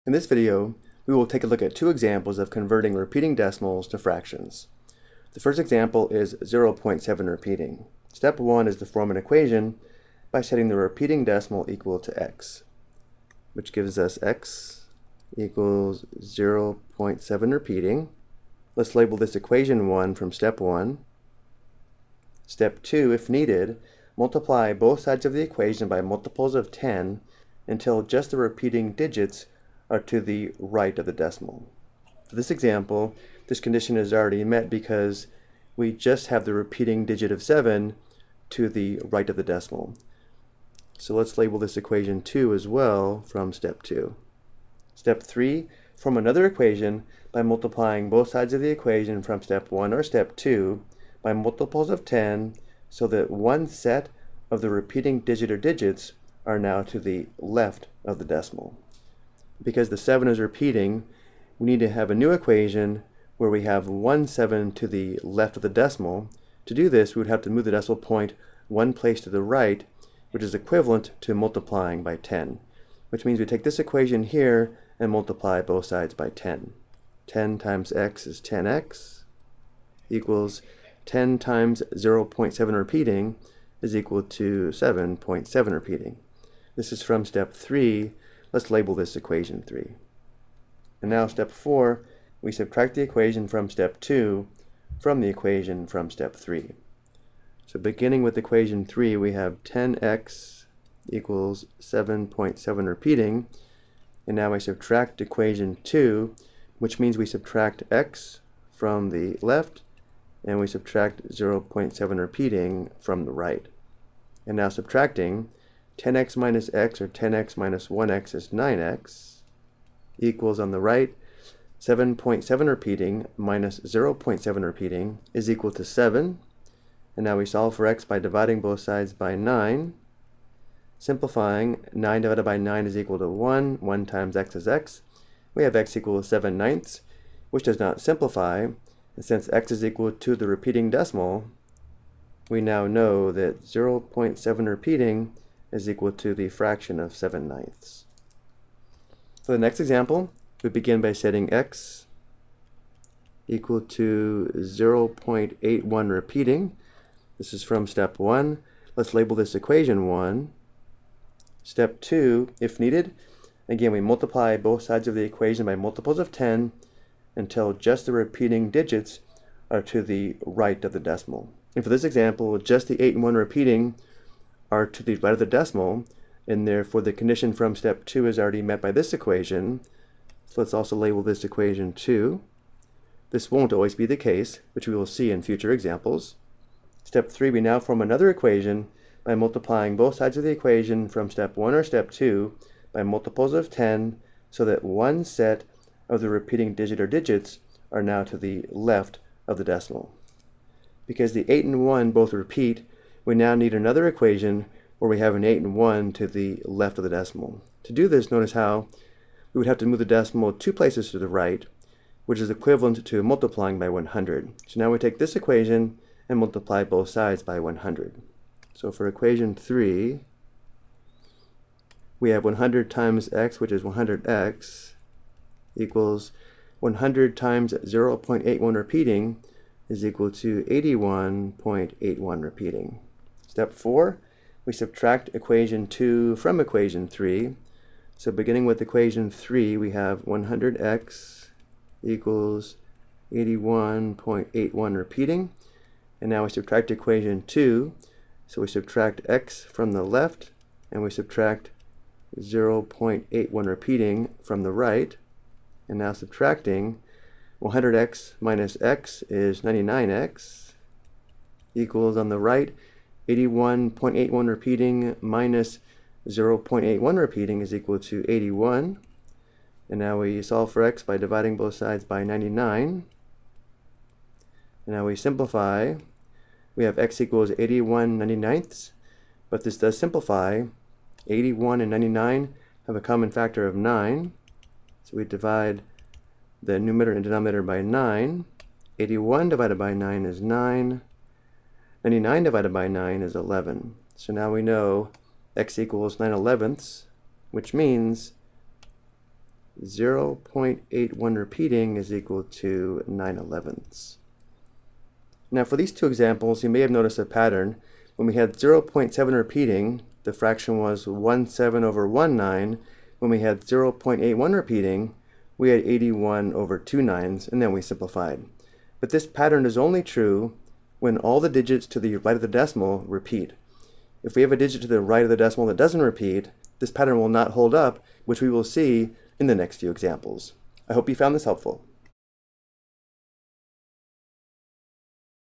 Convert a Repeating Decimal to a Fraction (Part 1)
This video explains how to convert a repeating decimal to a fraction.